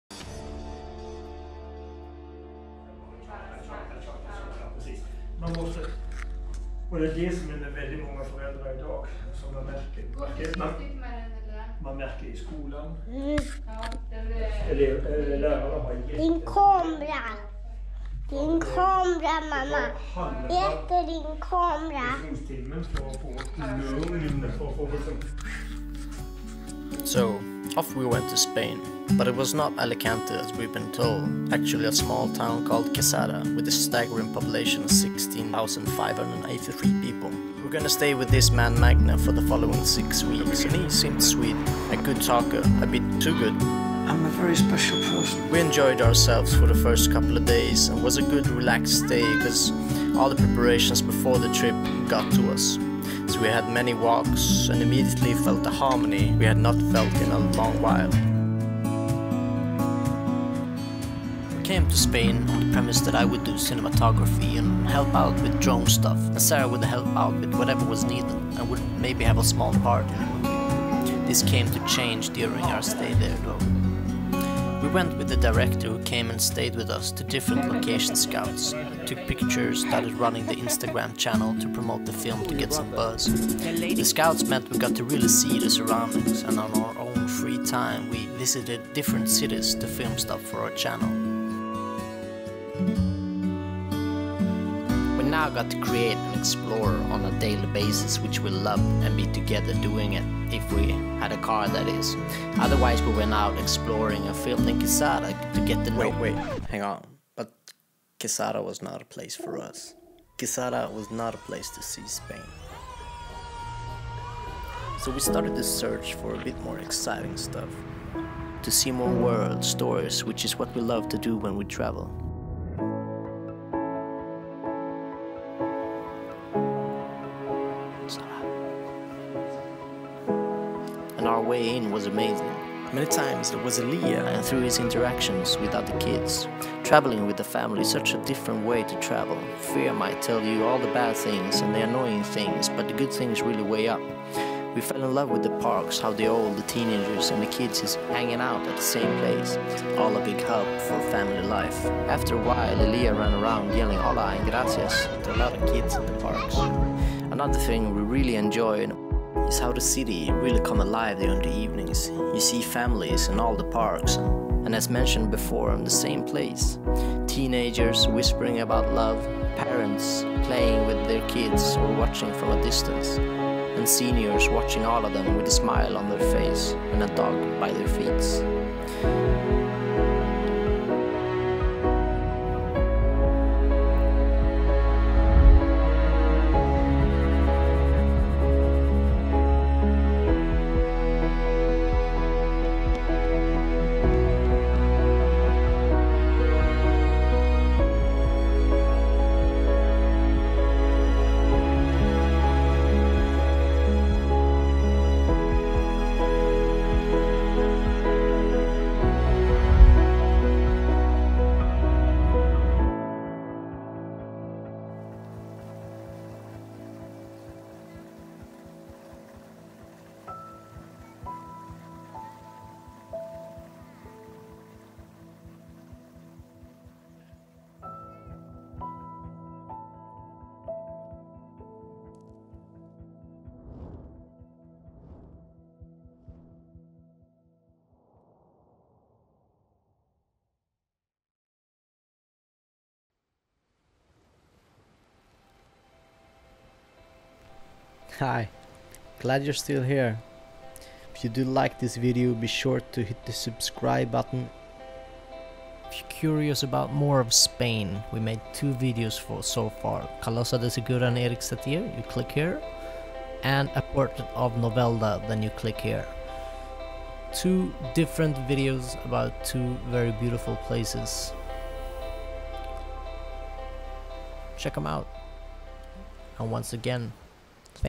Humble beginnings in Spain - We do love Spain.
Thoughts on Spain and insights to our stay

We spent 6 weeks in Spain around the Costa Blanca region. It was a good way to start our long trip. Spain really is a nice place, a place where I Could imagine growing old, sitting on some bench, hand in hand looking at some teenagers full of life and thinking back on mine.

The Idea was to work on a film production, the production got delayed a couple of weeks thou so we had to leave before the shooting began. We did some pre production work and social media work instead. Which was fun but not really what we signed up for so we were glad to go on the next adventure when the time came.

We filmed alot in different places when we were there, two videos are already up on our channel, just go check them out if you're curious.

In the pipeline there is more videos from Spain, then different kinds of films from Tobago and St Lucia of the Caribbean.

another_family_life

PEACE & LOVE
//Sarah, Alec and Eliah
//Another Family Life

PS. Gotta love that ending, sorry bout that but hey, mistakes can be cool. Mistakes is the new...?